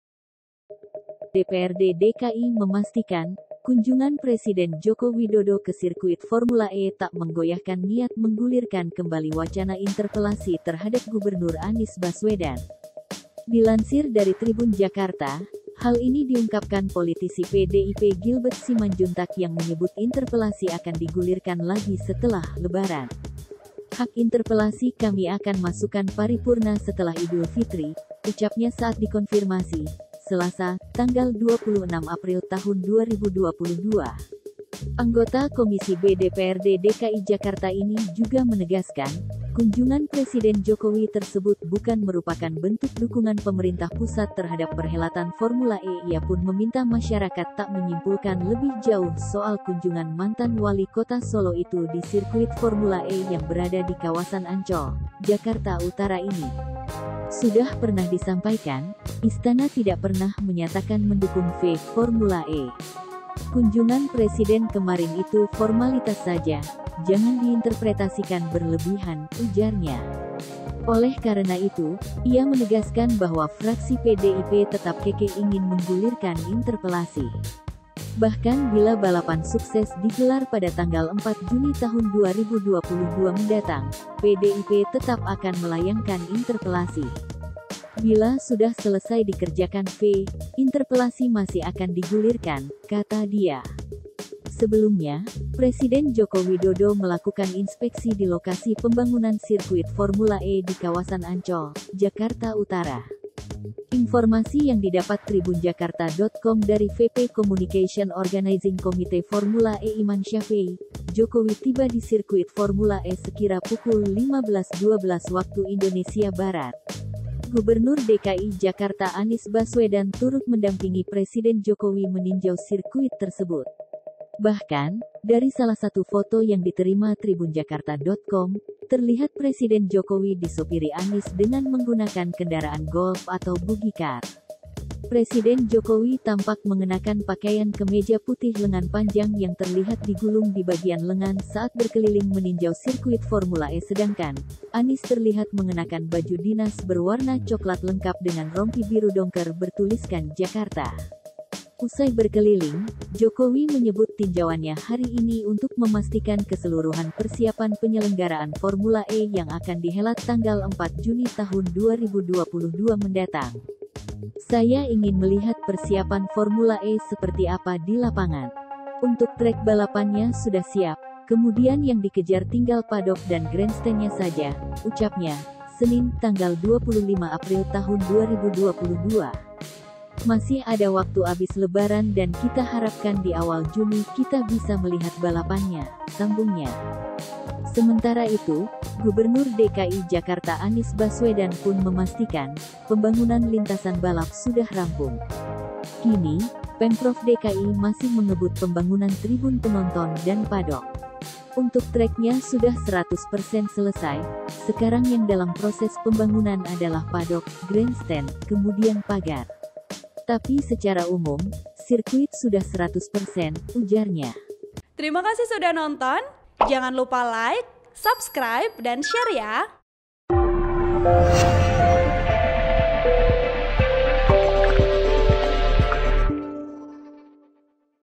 Kunjungan Jokowi ke Sirkuit Formula E Tak Pengaruh, PDIP Tetap Interpelasi Anies
Fraksi PDIP DPRD DKI memastikan, kunjungan Presiden Joko Widodo ke sirkuit Formula E tak menggoyahkan niat menggulirkan kembali wacana interpelasi terhadap Gubernur Anies Baswedan.
Dilansir dari Tribun Jakarta, hal ini diungkapkan politisi PDIP Gilbert Simanjuntak yang menyebut interpelasi akan digulirkan lagi setelah lebaran.
"Hak interpelasi kami akan masukan paripurna setelah Idulfitri," ucapnya saat dikonfirmasi, Selasa (26/4/2022).

Editor: Jojo